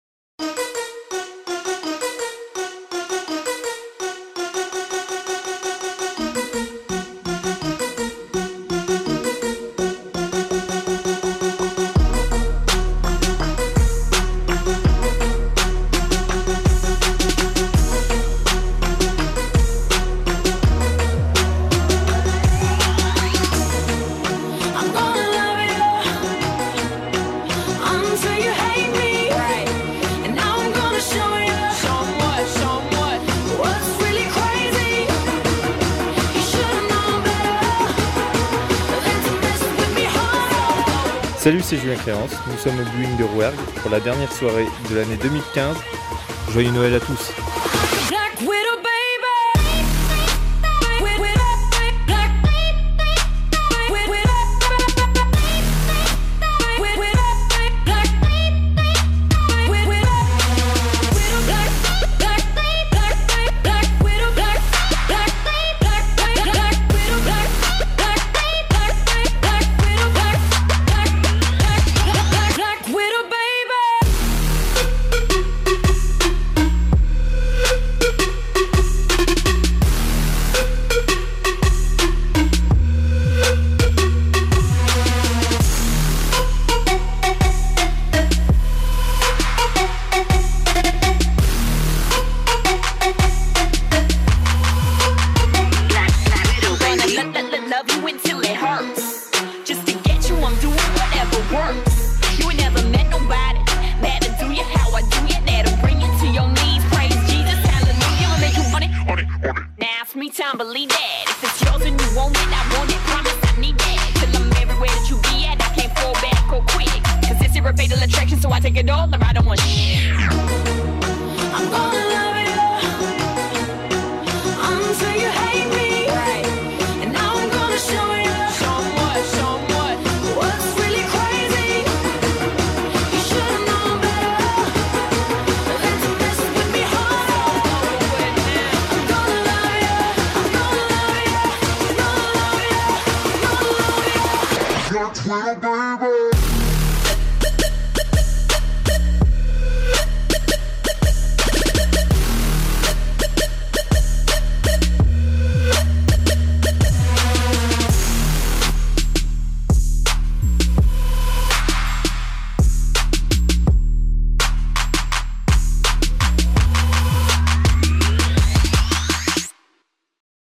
Bowling Du Rouergue Bastide - LAST NIGHT 2015
Voici l'After-movie de la soirée du 19 Décembre ! LAST NIGHT au Bowling Du Rouergue Bastide avec Julien CREANCE, Harold.M et Denis.V !
Un Grand merci à vous tous pour cette soirée qui restera une fois de plus dans les mémoires !